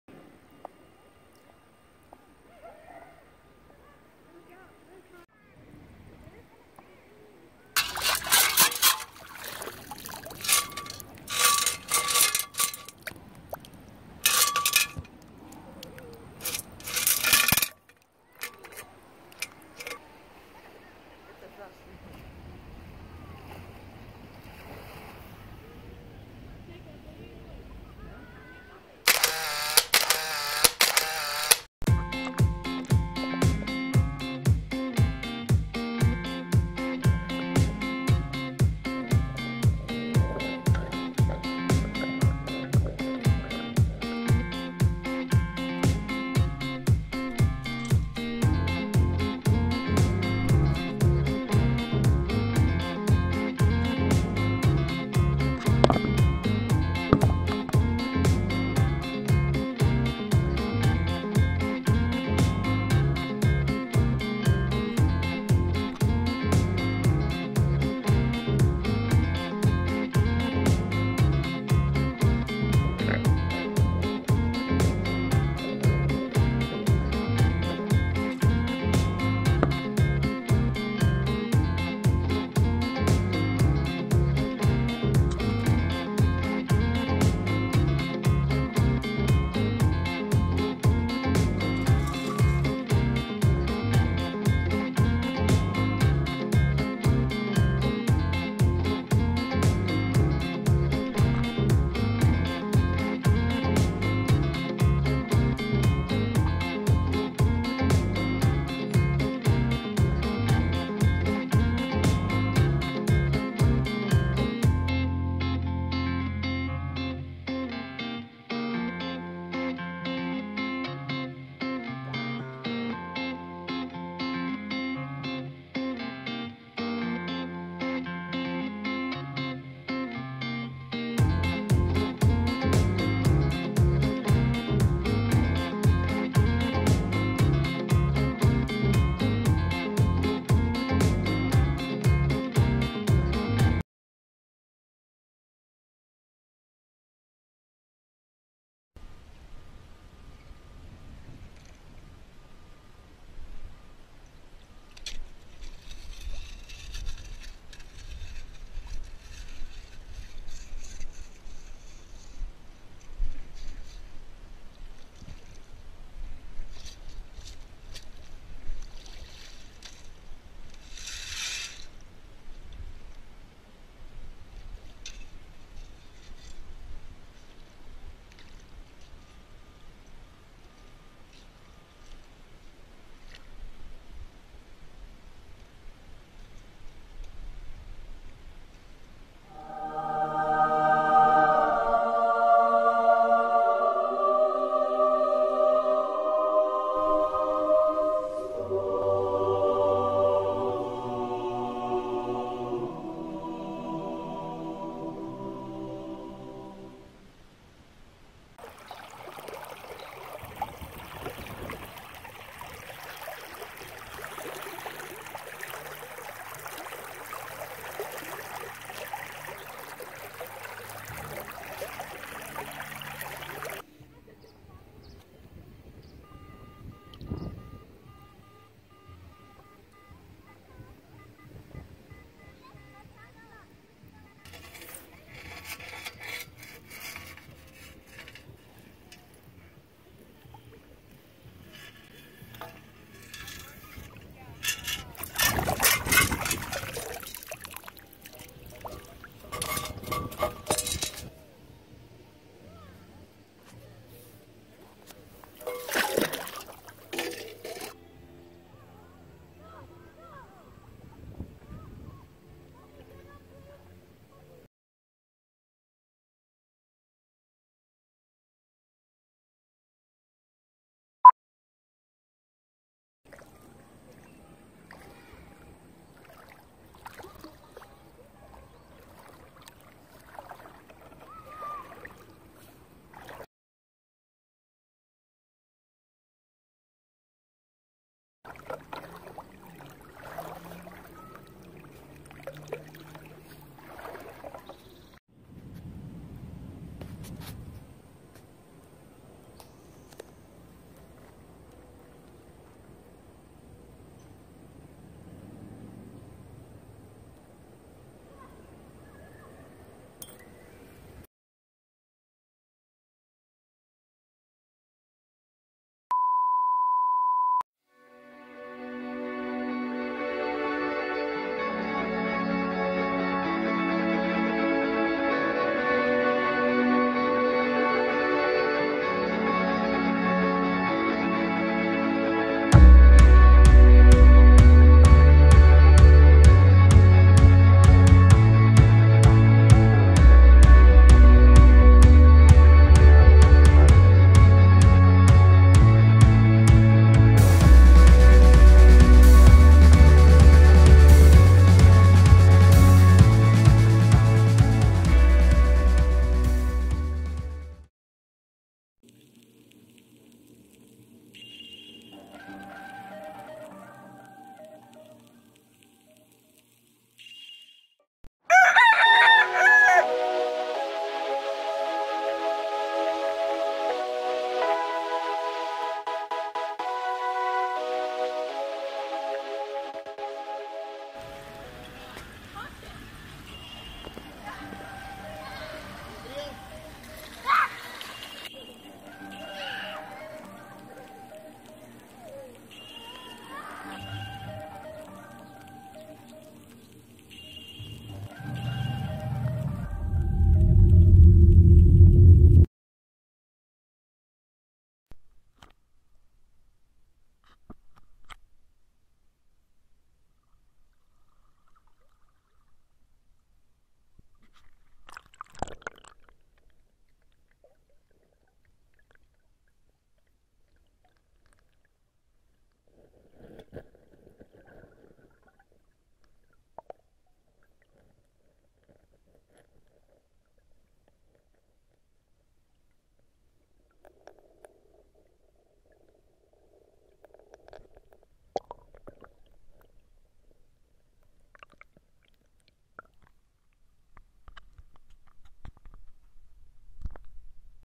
Metal Detecting: Compilation Of Various Random Clips
Just some various, random clips I've had laying around of some metal detecting finds.
Hope I remember how to upload, guess I'll find out!